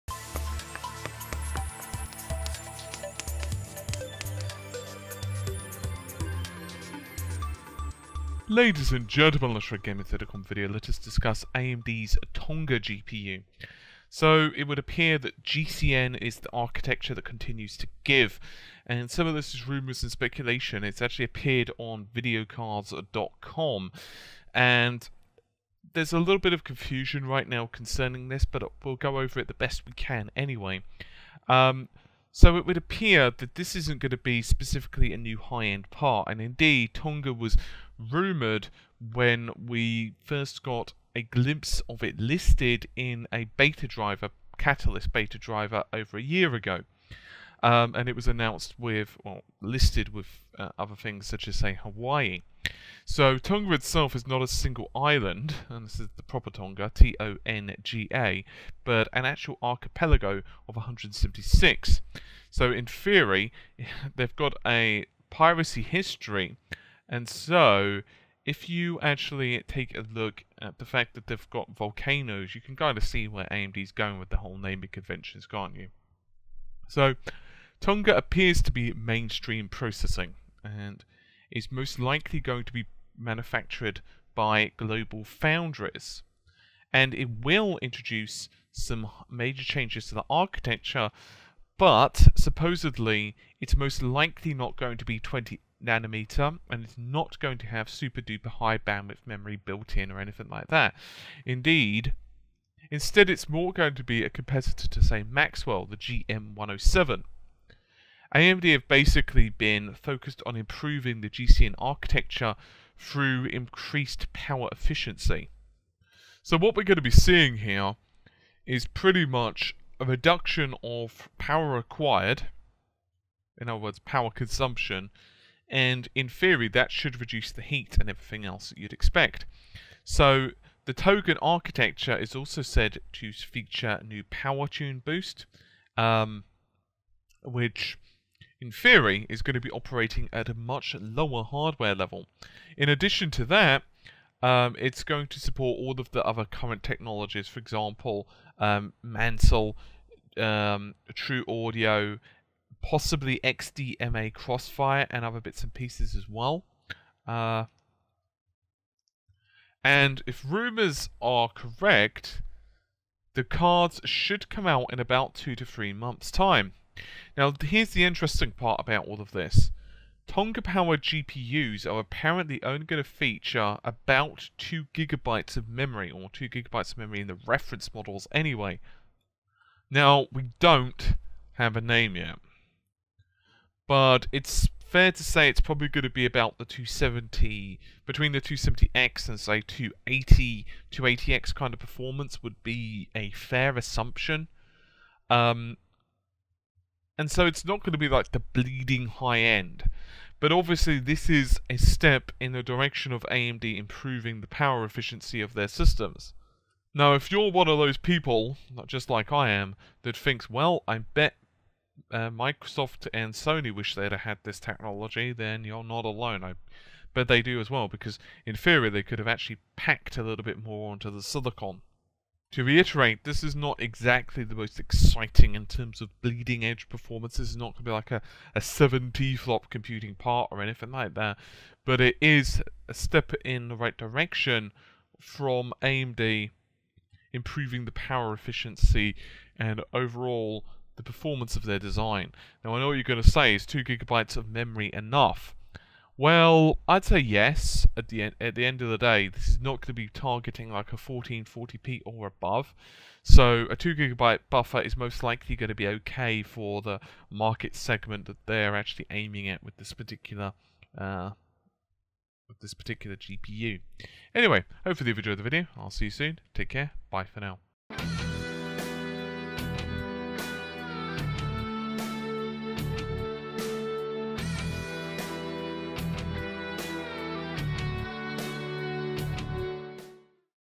AMD - New AMD GPU "Tonga" Rumored - Lower Power Consumption Lower Heat & More Efficient
AMD's next range of GPU's - the so called Tonga range are going to be released soon and much like Nvidia's maxwell focused on a reduction of power usage and improved thermals.